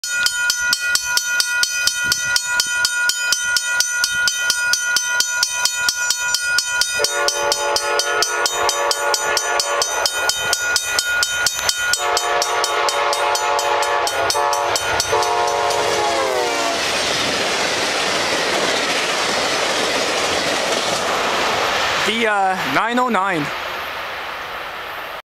very fast VIA 53 w/ VIA Engine 909 leads 7 cars thru Boundary Road in Newtonville
heres my 8th catch from Newtonville

and the only VIA i flimed. we caught alot but this was the only one i flimed.

flimed with- ‪admydragon, osrx1, ‪TheEtobicokeRailfan, ttcsubway209, trainfanjon, widecabfan & yurysausage..

video taken- Saturday April 9th 2011 at 11am
and as always, Comment & Enjoy !
thanks ! =]